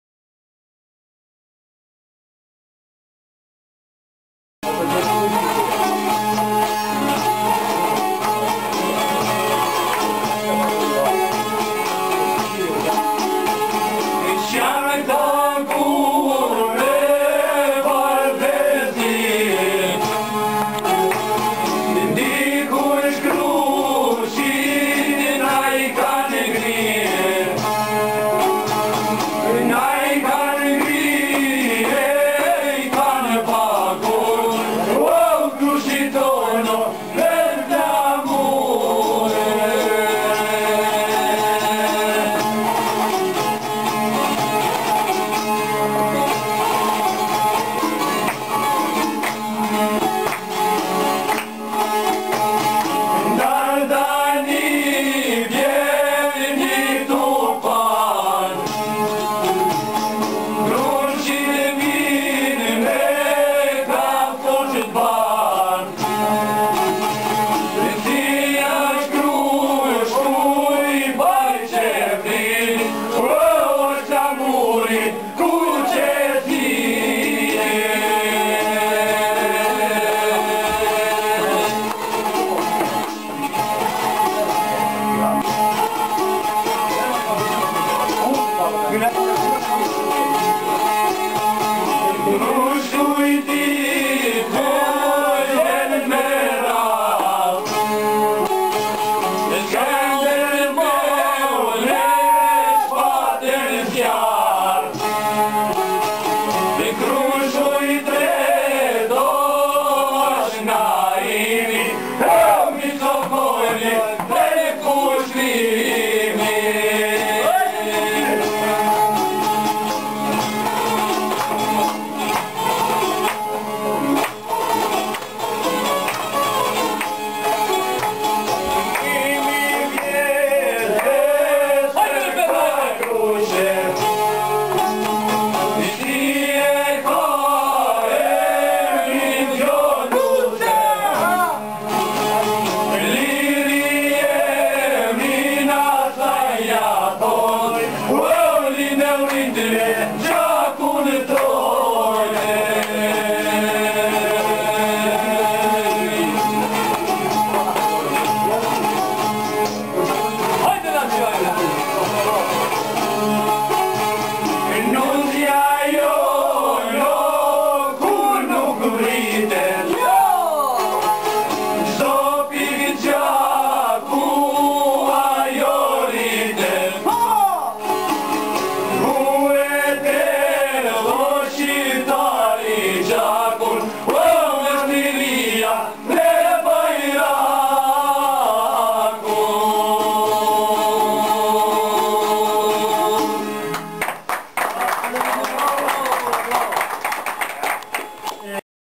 Përshefc-Veteranët e SHKA Jehona
prezanton momente nga ushtrimet që bëhen nga antarët e SHKA Jehona-Përshefc për organizimin e Odës së burave